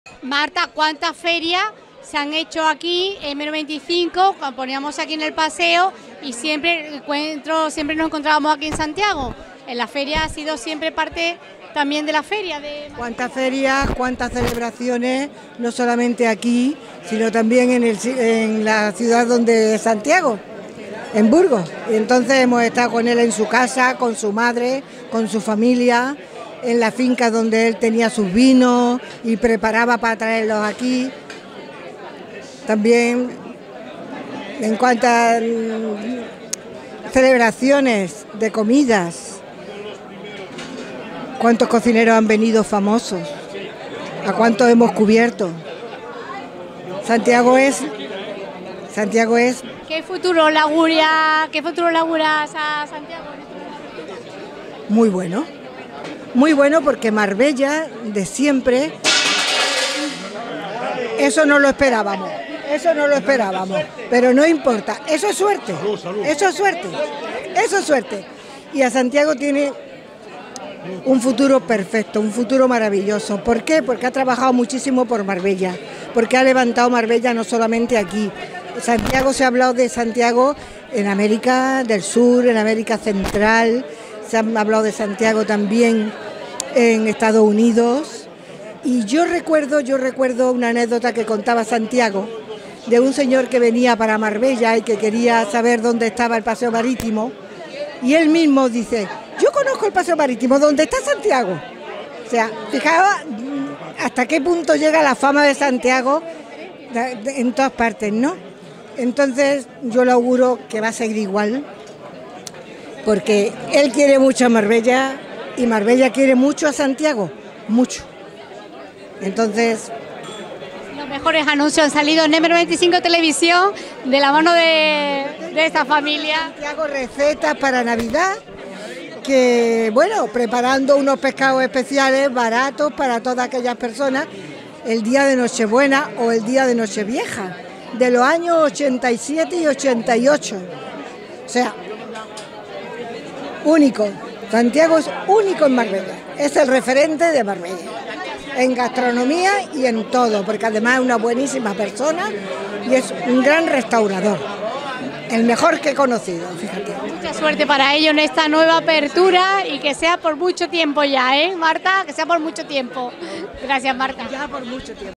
Entrevista de Helena OLaya a La Directora de M 95 TV Marta Ortega en la reapertura de Restaurante Sa
Entrevista de Helena OLaya a La Directora de M 95 TV Marta Ortega en la reapertura de Restaurante Santiago